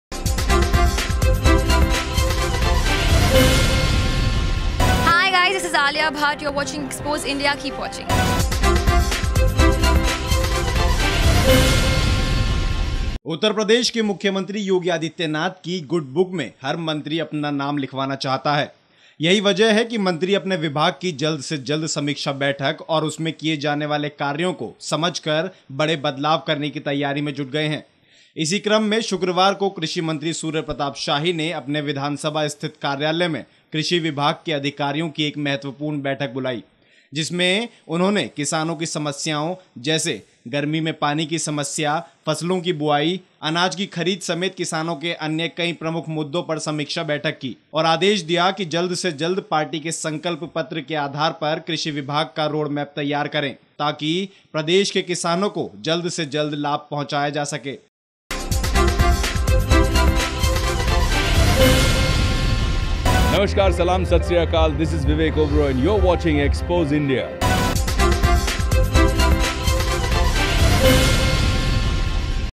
उत्तर प्रदेश सरकार में कृषि मंत्री सूर्य प्रताप शाही ने बुलाई बैठक
उत्तर प्रदेश के मुख्यमंत्री योगी आदित्यनाथ की गुड बुक में हर मंत्री अपना नाम लिखवाना चाहता है। यही वजह है कि मंत्री अपने विभाग कि जल्द से जल्द समीक्षा बैठक और उसमें किए जाने वाले कार्यों को समझ कर बड़े बदलाव करने की तैयारी में जुट गए हैं। इसी क्रम में शुकवार को कृषि मंत्री सूर्य प्रताप शाही ने अपने विधानसभा स्थित कार्यालय में कृषि विभाग के अधिकारियों की एक महत्वपूर्ण बैठक बुलाई जिसमें उन्होंने किसानों की समस्याओं जैसे गर्मी में पानी की समस्या, फसलो की बुवाई, अनाजों की खरीद समेत किसानों के अन्य कई प्रमुख मुद्दों पर समीक्षा बैठक की और सभी अधिकारीयो को निर्देश दिया कि जल्द से जल्द पार्टी के संकल्प पत्र के आधार पर कृषि विभाग का रोड मैप तैयार करें ताकि प्रदेश के किसानों को जल्द से जल्द लाभ पहुंचाया जा सके।